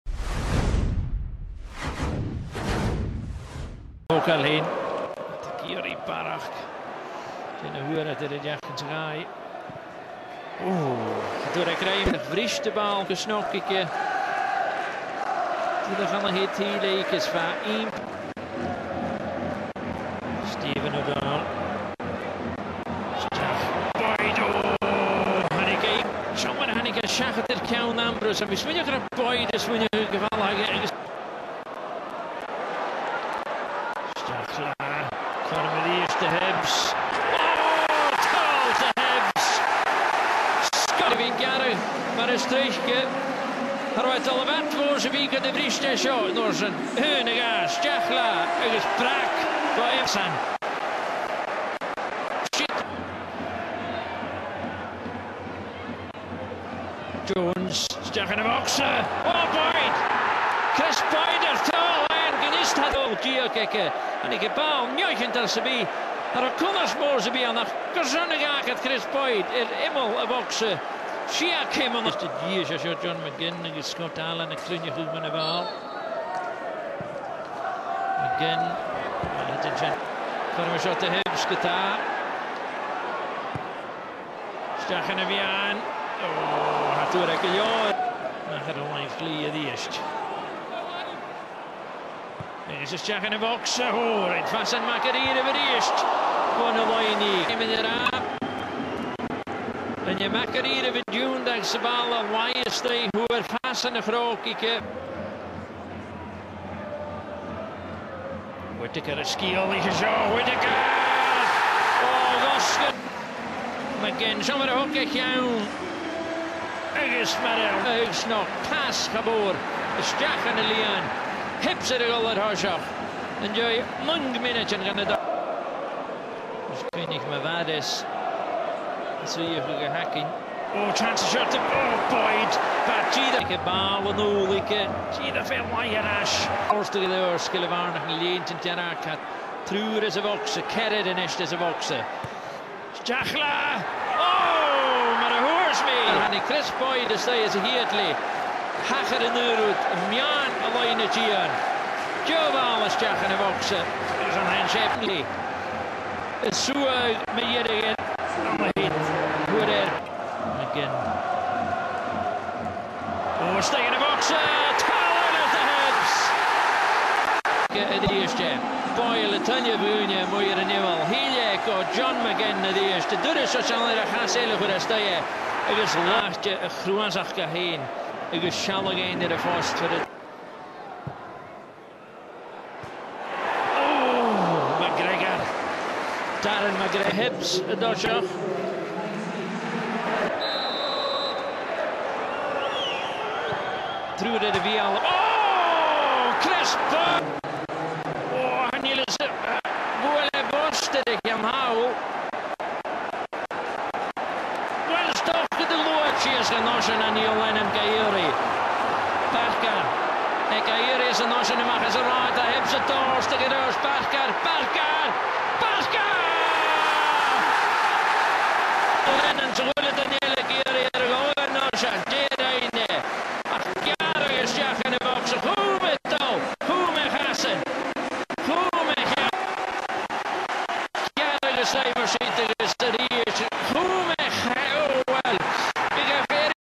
Hibernian - Kilmarnock 5-3 28-04-2018 Highlights Scottish Premiership
Scottish Premiership 2017/18
Hibernian 5 - 3 Kilmarnock

goals
1 - 0 Scott Allan 29'
1 - 1 Kris Boyd 33'
2 - 1 Steven Whittaker 54'
2 - 2 Stuart Findlay 61'
3 - 2 Jamie Maclaren 64'
4 - 2 Florian Kamberi 76'
4 - 3 Kris Boyd 80'
5 - 3 Brandon Barker 90+2'